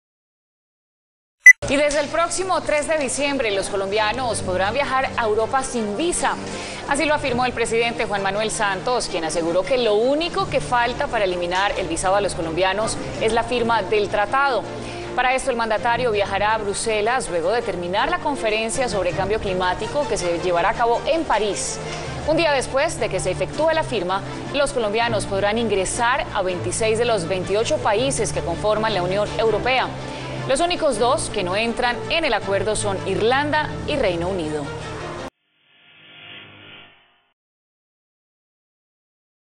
Citytv: Confirman eliminación de visa Schengen a colombianos
A partir del 3 de diciembre ya no será necesaria la visa para ingresar a 26 de los 28 países de la Unión Europea.

Emisión: 28 de octubre de 2015